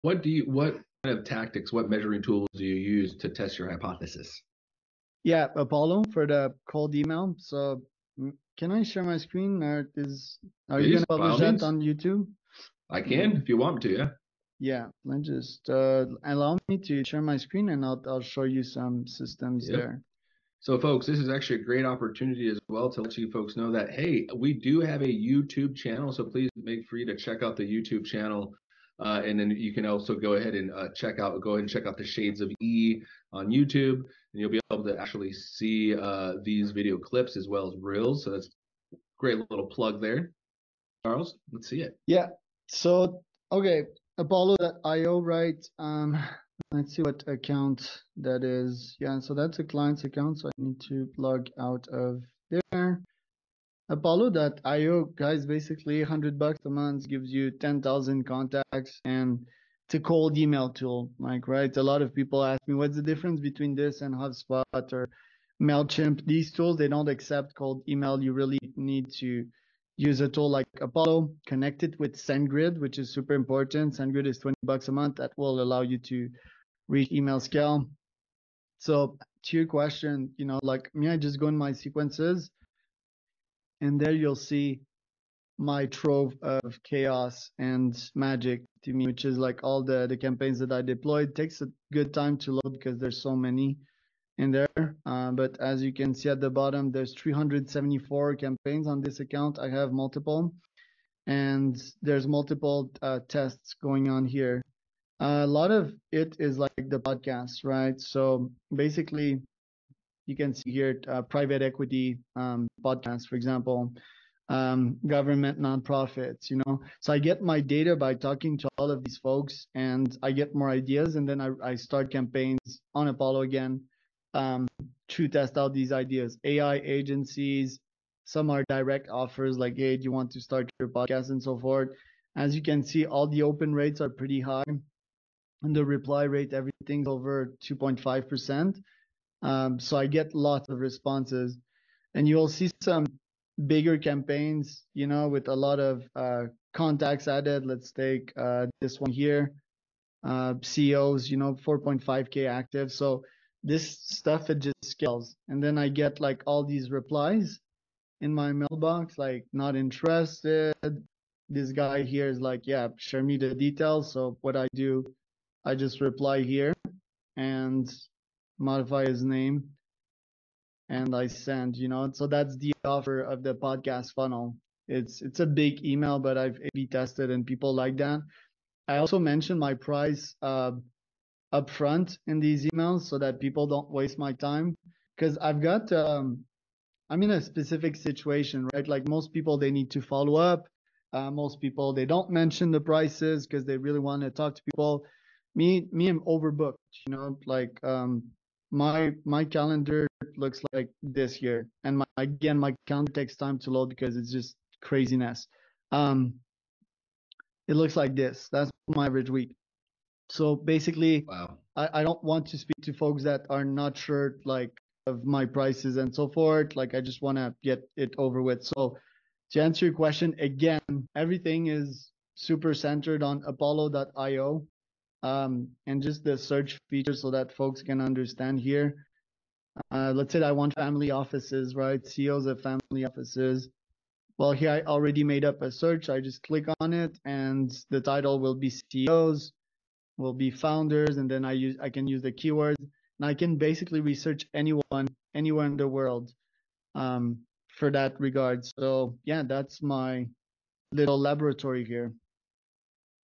Charles Cormier's laboratory on The Shades of Entrepreneurship
Charles discussed his use of Apollo.io as a cold email tool, mentioning its benefits over other tools like HubSpot or MailChimp. He highlighted his extensive campaign deployment, high open and reply rates, and the efficiency of mentioning prices upfront in his emails.